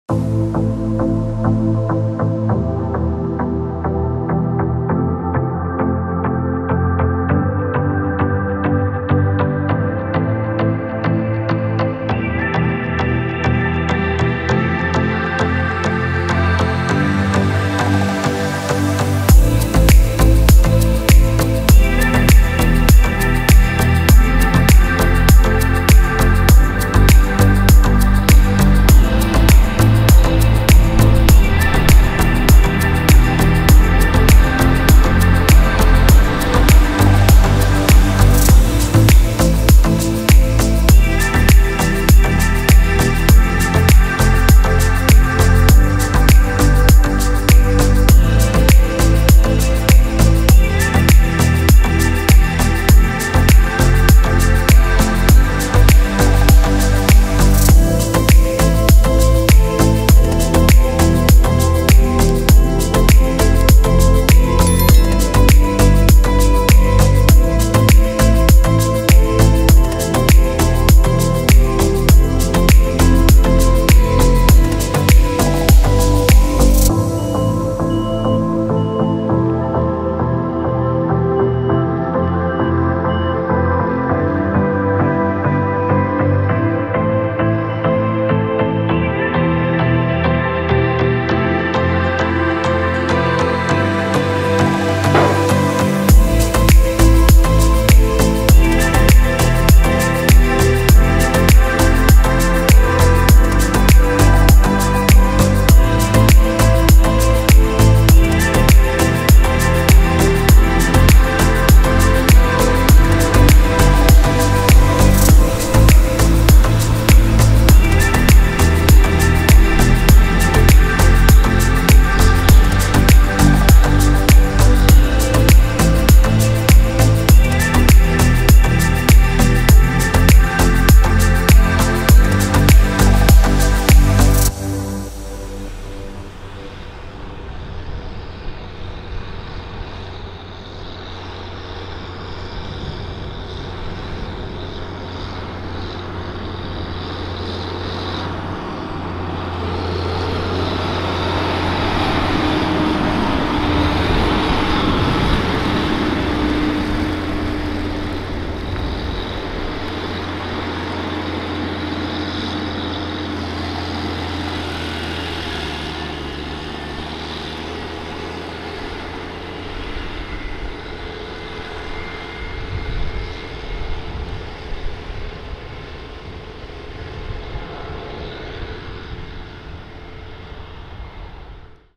Three wheel fertilising with a CHALLENGER TERRA GATOR
Eastern Spreading putting out some sugar beet fertiliser with their Challenger TerraGator TG8303

See more agricultural action from the UK on our Instagram and Facebook accounts - @AgriArtUK.

Filmed with a DJI Mini 3 Pro and Sony A7IV in Suffolk.